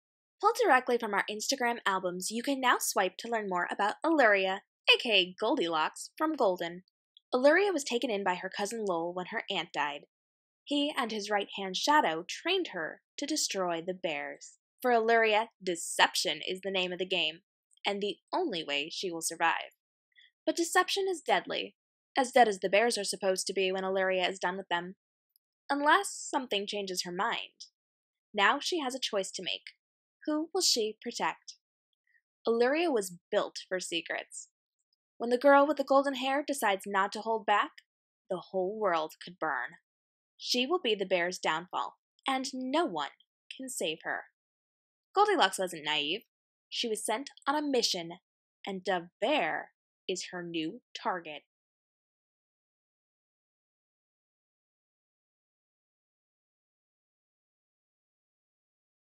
A Look At Auluria from The Golden Trilogy by K.M. Robinson
We pulled our Instagram Album about Auluria to give you a closer look at our Goldilocks from Golden by K.M. Robinson (Available from Snowy Wings Publishing March 28th)